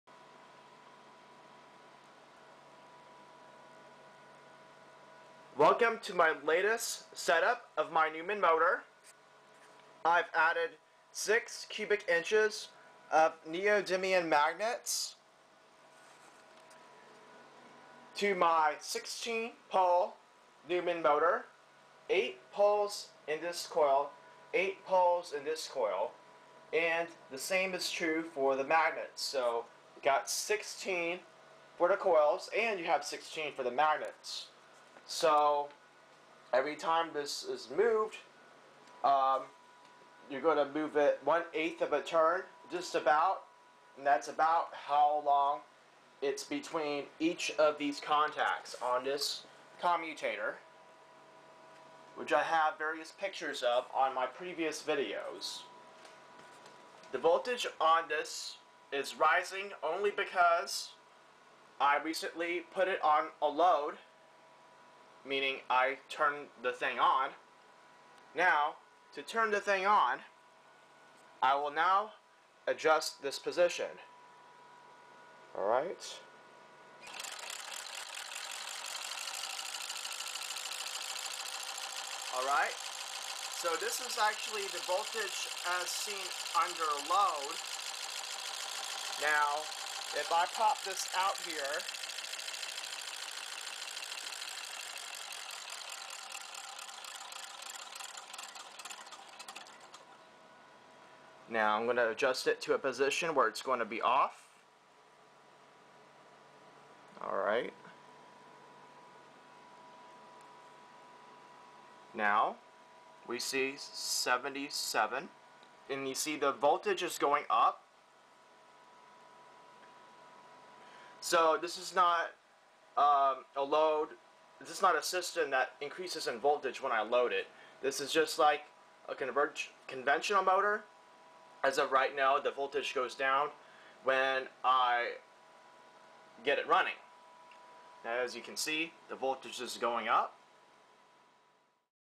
2011 Newman Motor (85V with no fan. 400 RPM)
(: 01215 = days since my first newman motor :)

(In the video I misstated the amount of magnets. I really added just 3 cubic inches, not 6.)

More magnets are to be shipped in just a few days. This will allow me to concentrate on the magnets toward the periphery. This will result in better performance than seen in this video. The way they are stacked now is too long along the axis, and it is creating problems with the lack of room on both ends of the shaft. My recent order should change that.